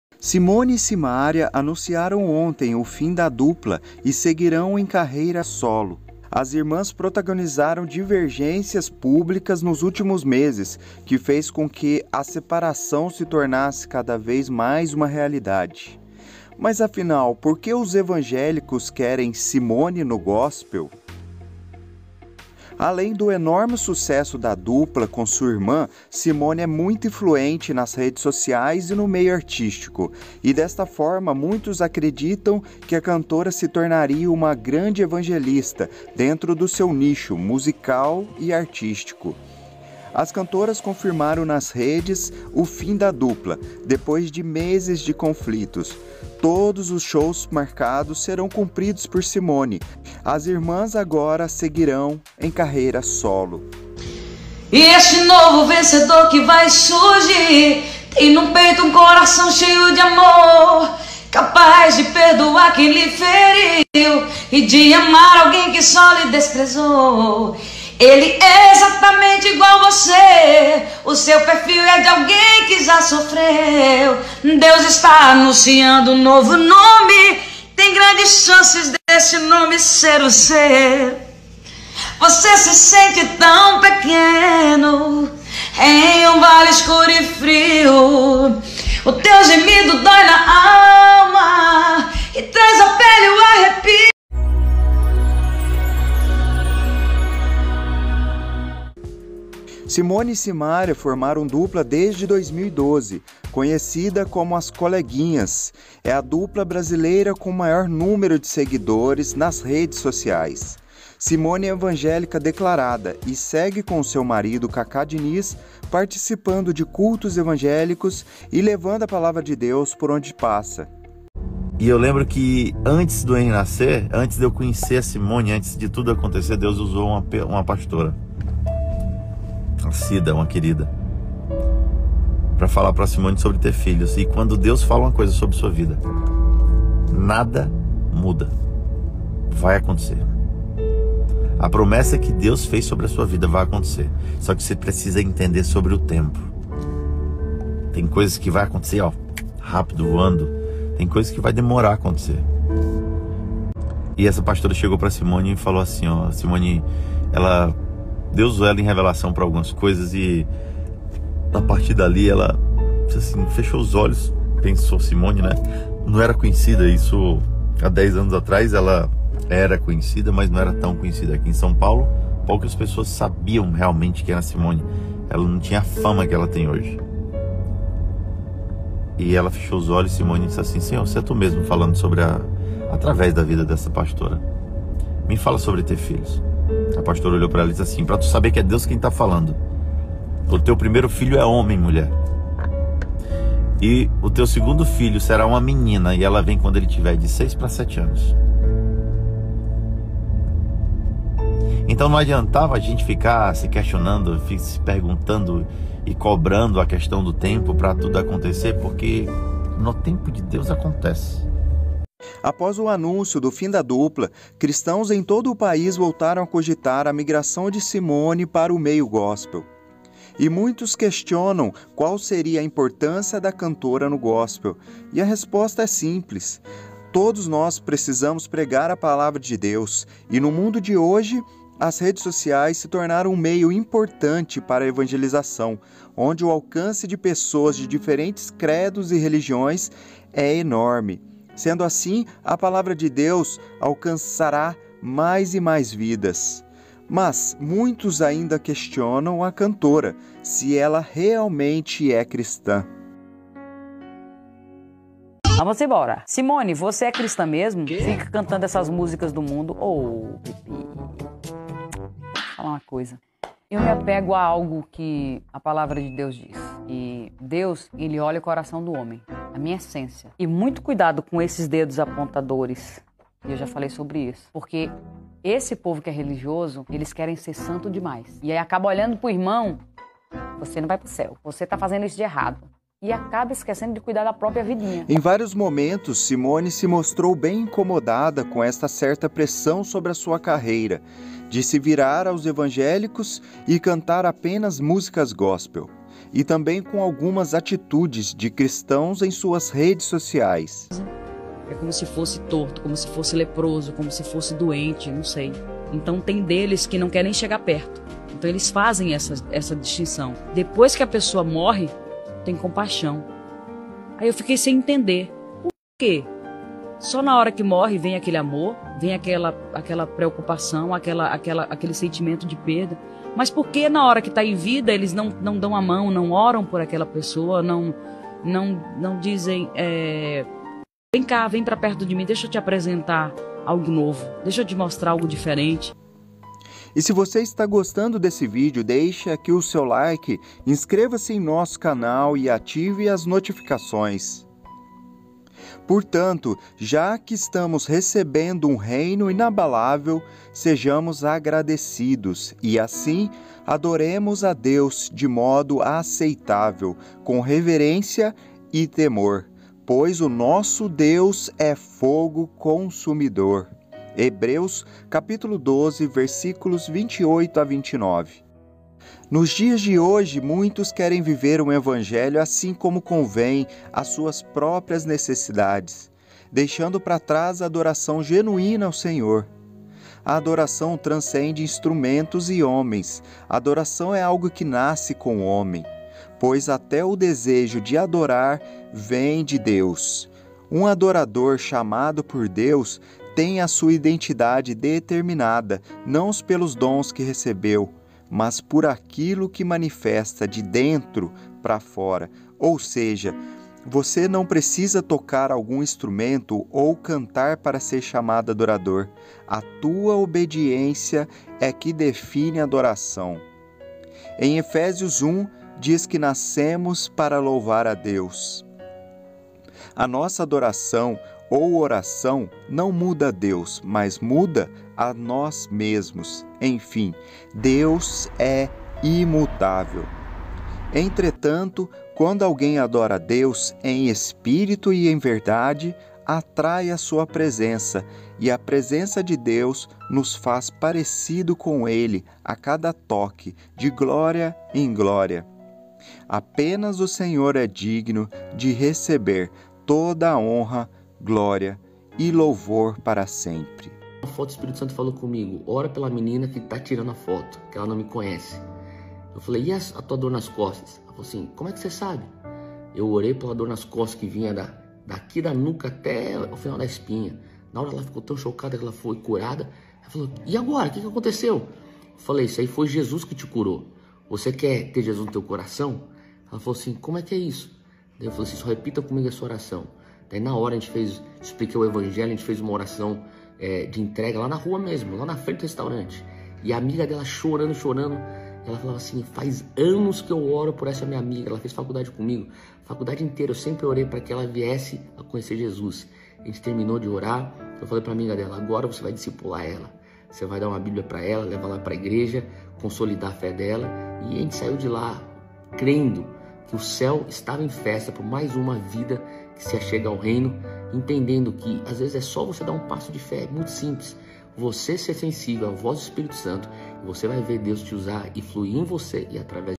SIMONE ESTÁ PERTO DO GOSPEL? Kaká Diniz da testemunho impactante!
Simone é evangélica declarada e segue com o seu marido Kaká Diniz participando de cultos evangélicos e levando a palavra de Deus!
Após o anúncio do fim da dupla, cristãos em todo o país voltaram a cogitar a migração de Simone para o meio gospel!
E muitos questionam qual seria a importância da cantora no gospel... E aí o que você acha de toda essa história?
🔥BEM-VINDO AO CLUBE!
🙌"Por isso, vistam toda a armadura de Deus, para que possam resistir no dia mau e permanecer inabaláveis"
Efésios 6:13
0:00 Clube da Palavra
0:05 Já deixou o seu LIKE no Vídeo?